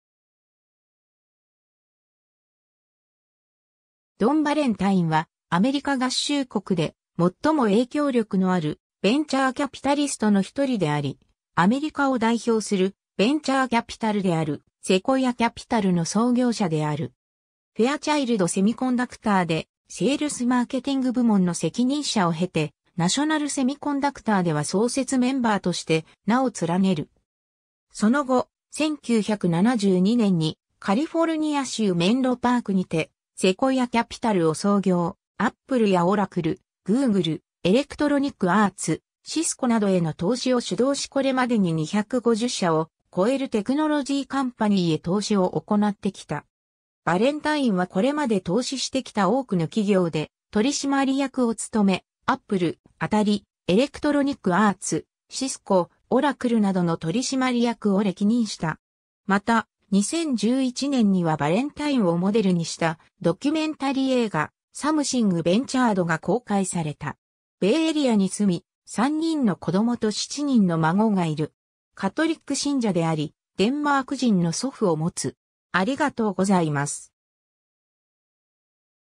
ドン・バレンタイン
ドン・バレンタイン（ドナルド・T・ドン・バレンタイン、Donald T. "Don" Valentine、1932年6月26日 - 2019年10月25日）は、アメリカ合衆国で最も影響力のあるベンチャー・キャピタリストの一人であり、アメリカを代表するベンチャーキャピタルであるセコイア・キャピタルの創業者である。
 フェアチャイルド・セミコンダクターでセールス・マーケティング部門の責任者を経て、ナショナル・セミコンダクターでは創設メンバーとして名を連ねる。
その後、1972年にカリフォルニア州メンロパークにてセコイア・キャピタルを創業、Appleやオラクル、グーグル、エレクトロニック・アーツ、シスコなどへの投資を主導しこれまでに250社を超えるテクノロジー・カンパニーへ投資を行ってきた。
 バレンタインはこれまで投資してきた多くの企業で取締役を務め、Apple 、アタリ、エレクトロニック・アーツ、シスコ、オラクルなどの取締役を歴任した。
また、2011年にはバレンタインをモデルにしたドキュメンタリー映画「Something Ventured」が公開された。
 ベイエリアに住み、3人の子供と7人の孫がいる。
カトリック信者であり、デンマーク人の祖父を持つ。